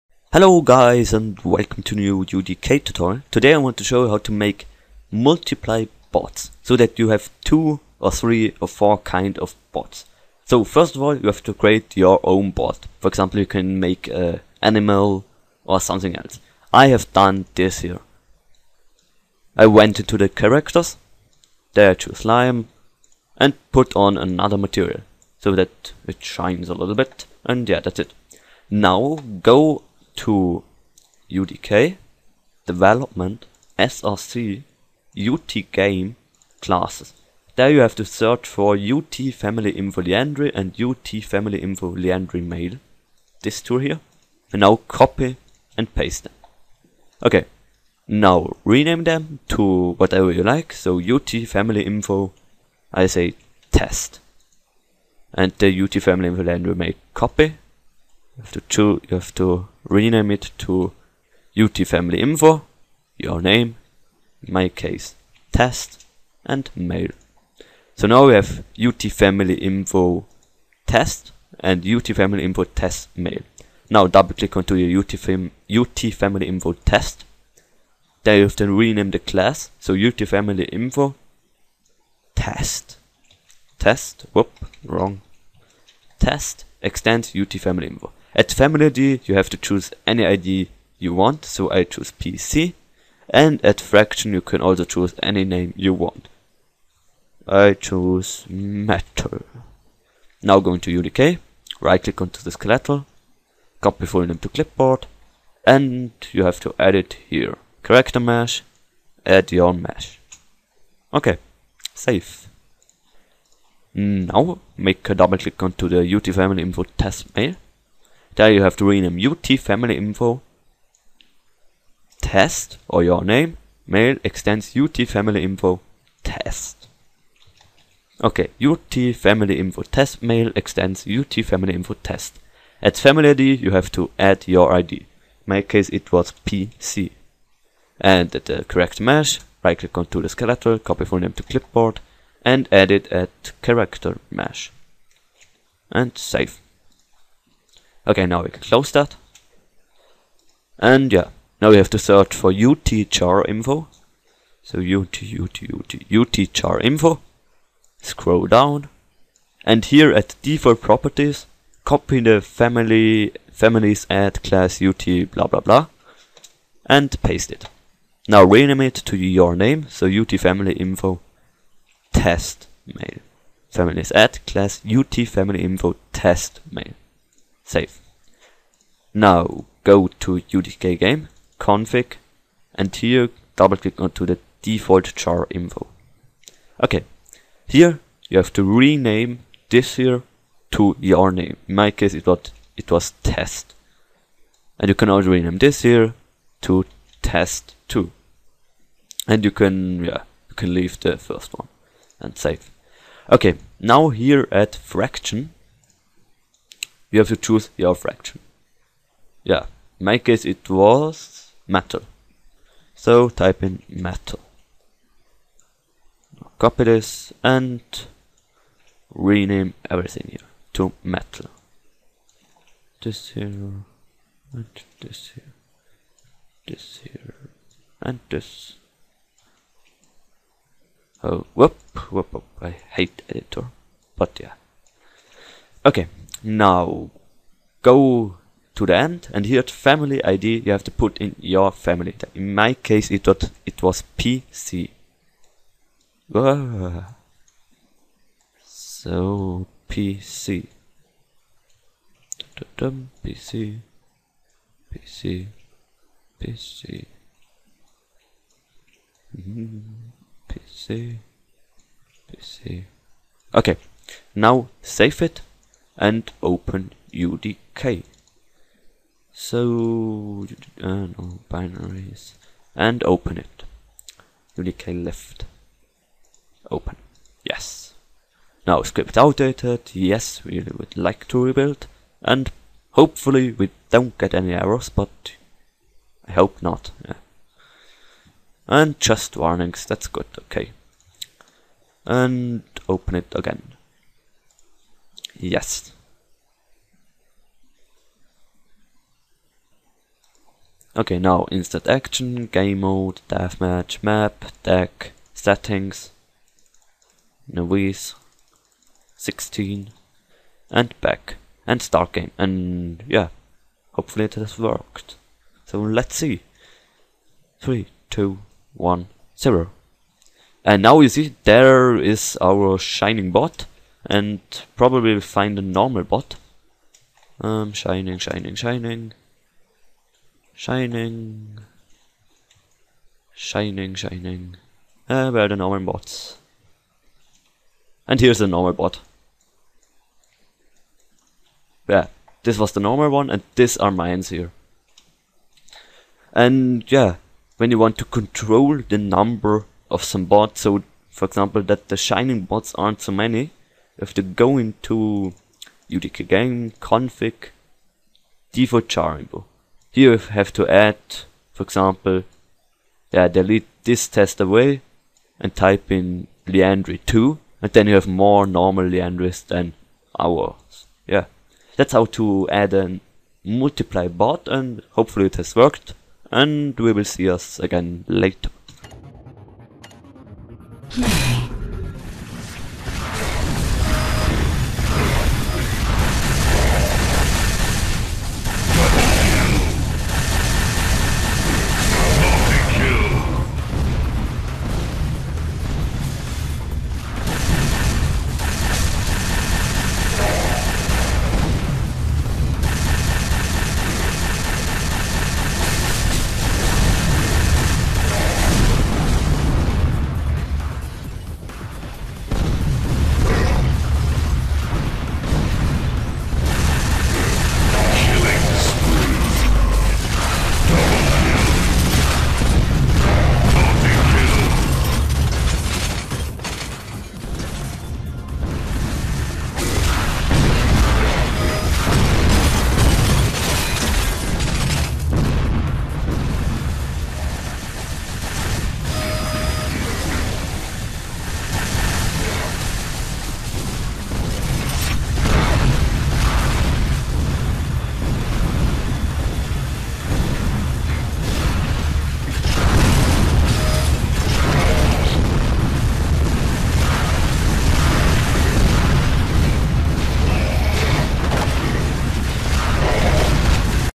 UDK Tutorial: multiple bots (english)
Today i show you how to make different bots.